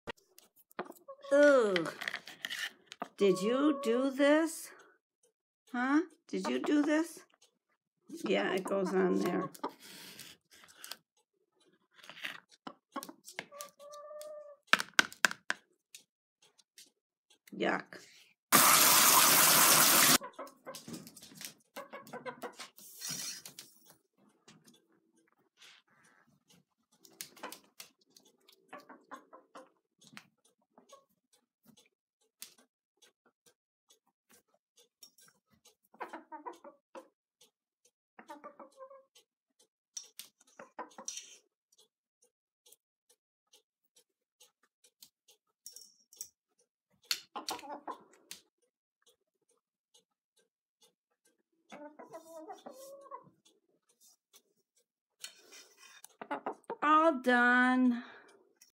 Poop Did I Do That
the look on Mr. Brown's face, Poop did I do that. Mr Brown the house hen pooped and has to help mop the floor. Enjoy watching Mr. Brown. this is a NonnaGrace